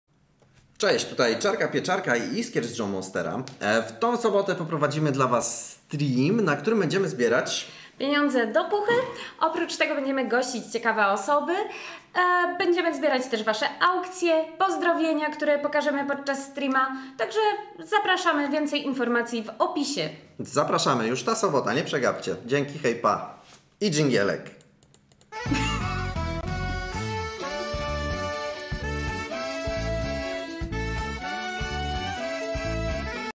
Zaproszenie na stream WOŚP - zbieramy do eSkarbonki Joe Monster
To już w tę sobotę, dzień przed 30. finałem WOŚP!

Patronat techniczny: SW Research

Plan streamu:
- Wywiady z gośćmi
- Gry i zabawy (też na ZOOMie)
- Emocjonowanie się wpłatami do eSkarbonki
- Przegląd aukcji, które wystawiają Bojownicy
- Rozmowy z widzami
- Dodatkowe atrakcje niespodzianki finansowe!